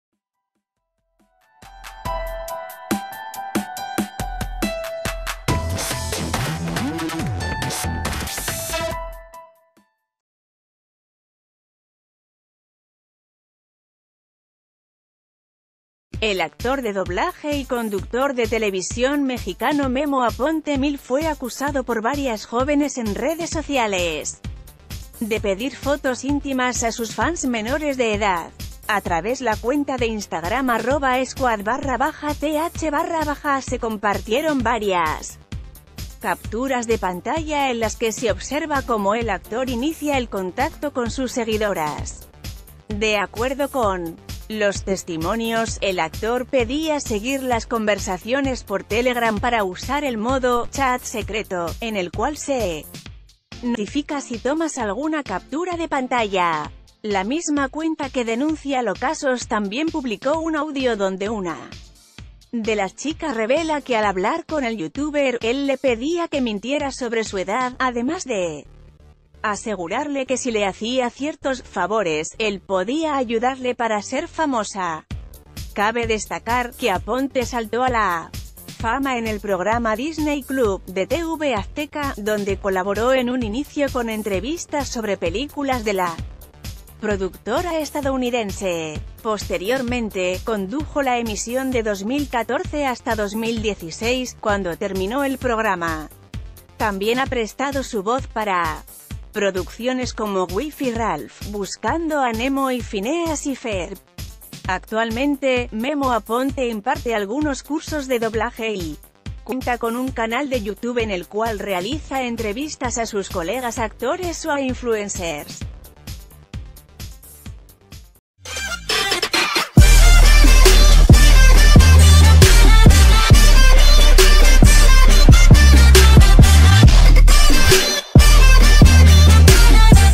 Acusan al youtuber Memo Aponte de pedir fotos íntimas a menores de edad
A través la cuenta de Instagram @squad_th_ se compartieron varias capturas de pantalla en las que se observa cómo el actor inic...